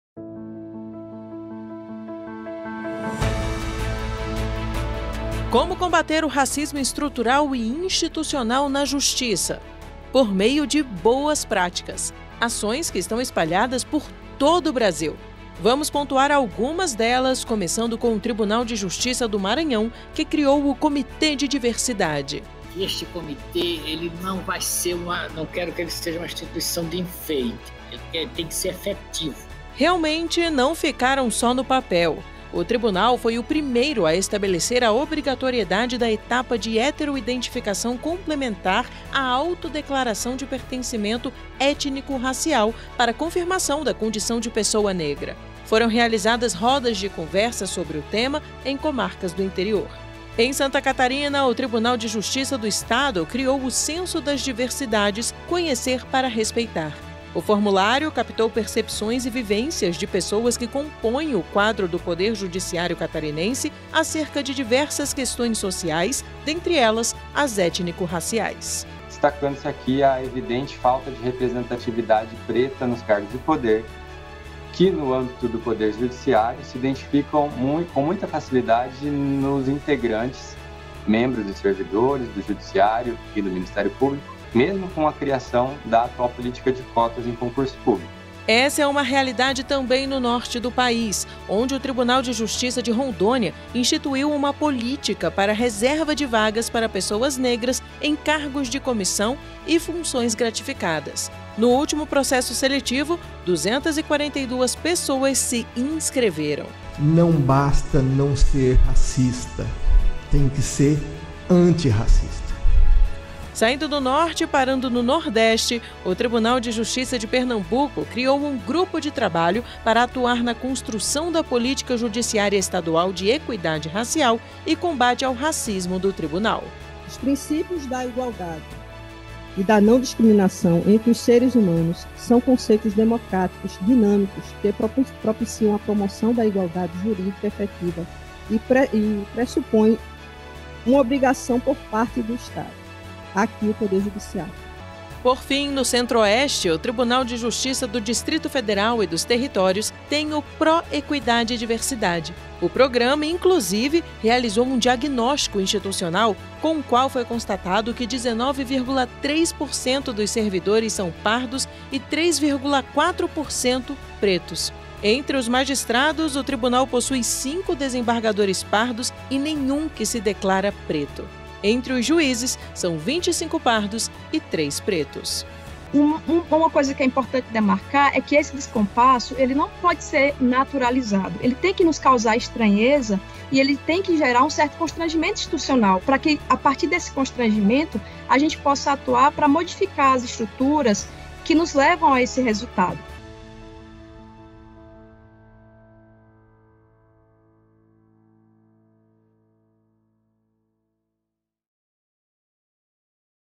Ações de tribunais estaduais de combate e enfrentamento ao racismo no Poder Judiciário
Para celebrar o Mês da Consciência Negra, o Superior Tribunal de Justiça (STJ) participa da 1ª Jornada Justiça e Equidade Racial: valorizando raízes, transformando futuros. O evento ocorrerá entre os dias 13 e 28 deste mês, com uma série de palestras, debates e ações em prol da equidade racial. A jornada conta com a participação do Supremo Tribunal Federal (STF), de todos os tribunais superiores, do Conselho Nacional de Justiça (CNJ) e do Conselho da Justiça Federal (CJF).
Durante o seminário no STJ, foi exibido um vídeo, produzido pela Secretaria de Comunicação Social do Superior Tribunal de Justiça, que destaca as ações de combate e enfrentamento ao racismo de alguns tribunais estaduais.
Assista também ao vídeo que destaca as ações do STF, CNJ e Tribunais Superiores: